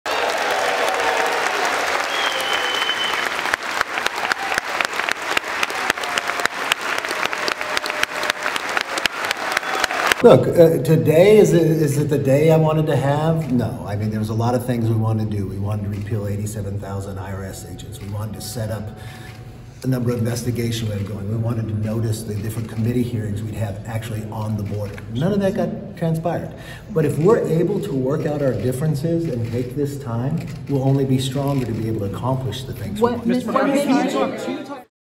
House adjourns as Republican McCarthy fails in marathon votes for speaker role
Republican leader Kevin McCarthy has fallen far short in voting to become speaker of the US House of Representatives.